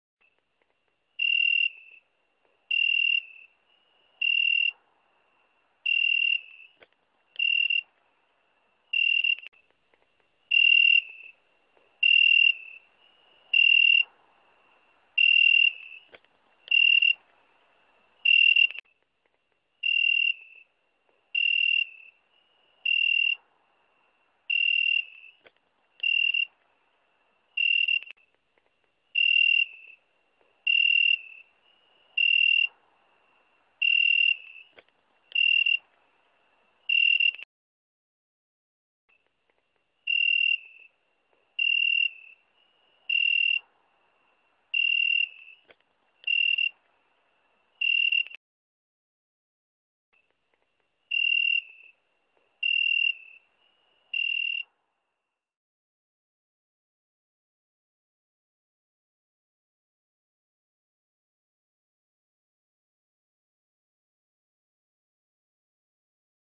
Zrikavac Cricket Siroki 2007
Zrikavac nije skakavac niti cvrčak. Ljeti navečer pušta ugodne zvukove. Od skakavca se razlikuje duzinom antena i ima dublji ton. Recorded in Široki Brijeg.
A 'zrikavac' is not a grasshopper or a cricket. In the summer evenings it makes pleasant sounds. It differs from the grasshopper in the length of its antennae and has a deeper tone. Recorded in Široki Brijeg.